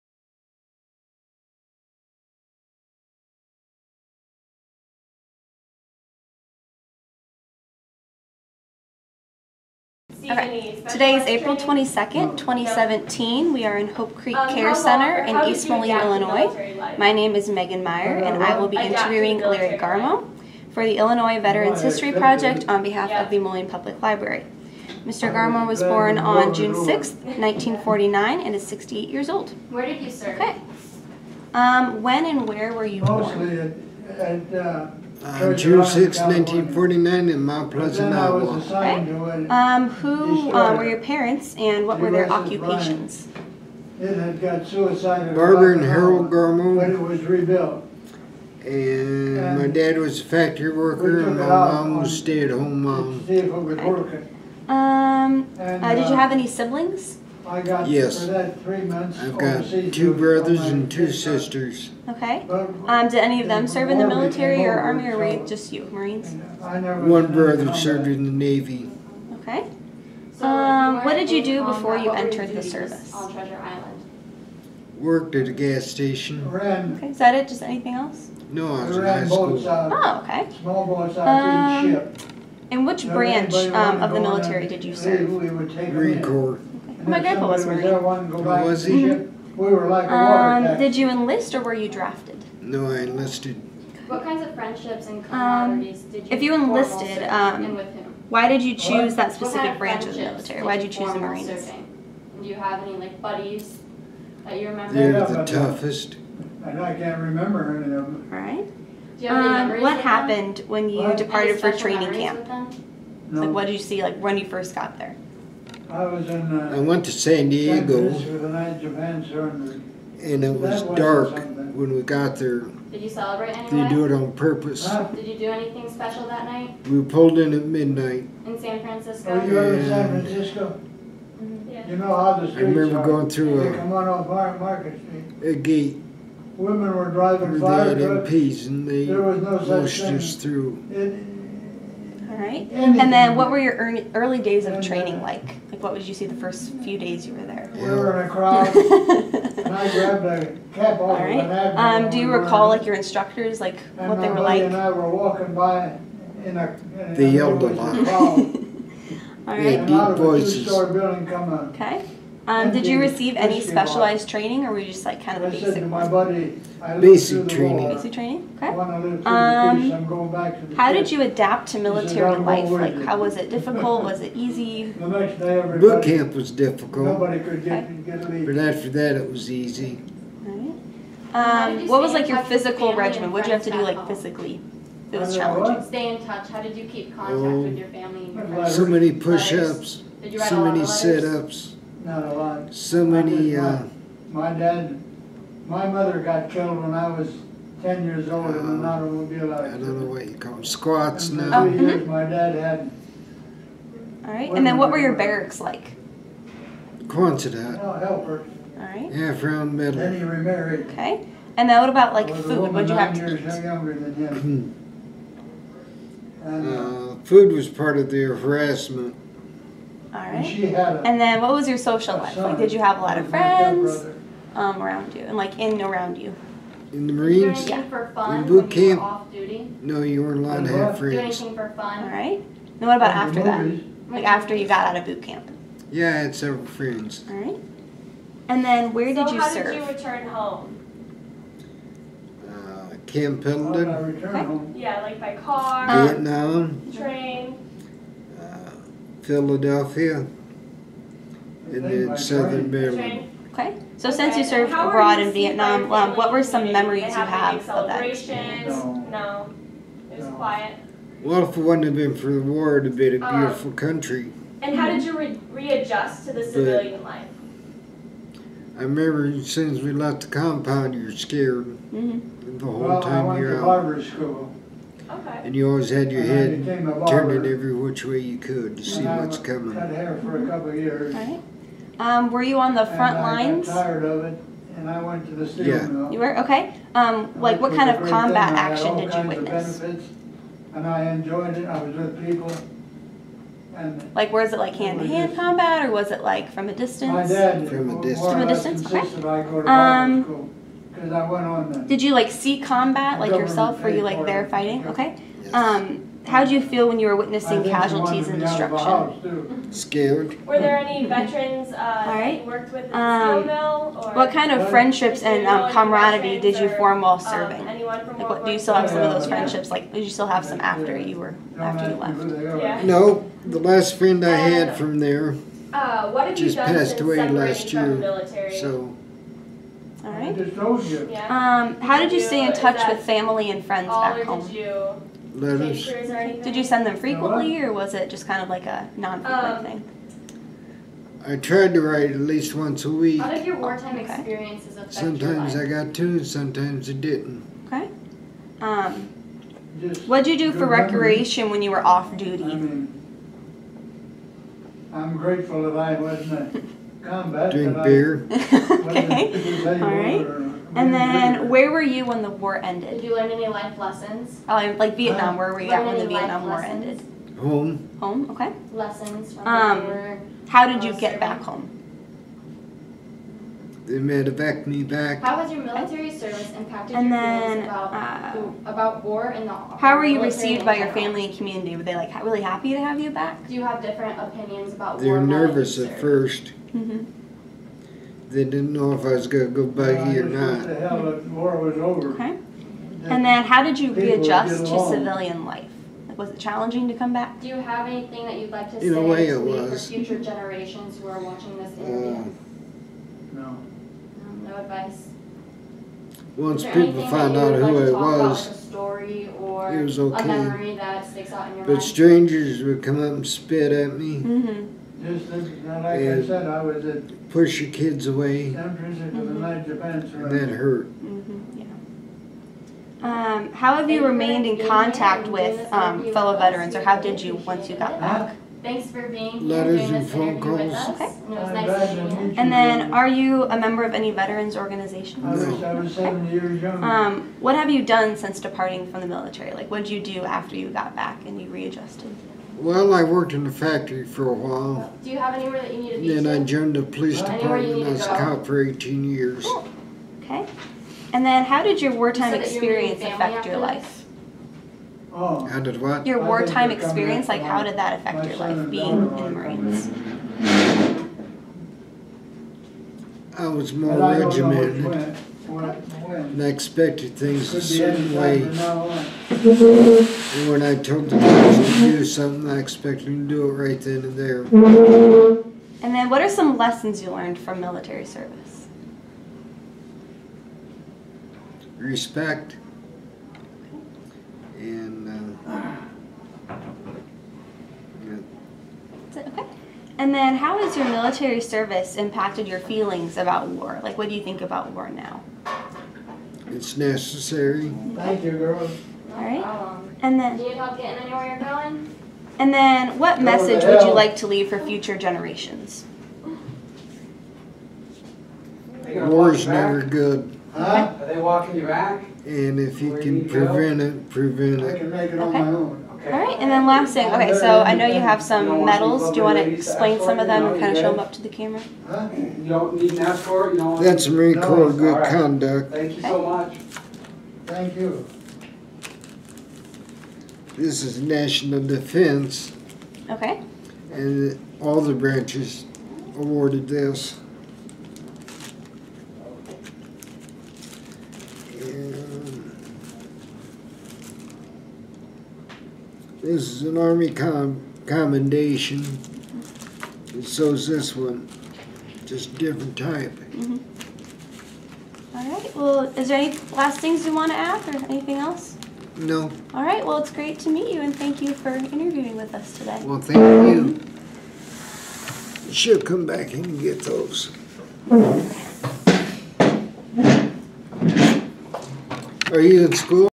Oral History Interview with Larry D. Garmoe -- Illinois Veterans History Project
Mr. Garmoe served in the United States Marines during Vietnam & Desert Storm from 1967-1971.  Interview conducted on April 22, 2017.  Illinois Patriot Form is available via the Illinois Digital Archives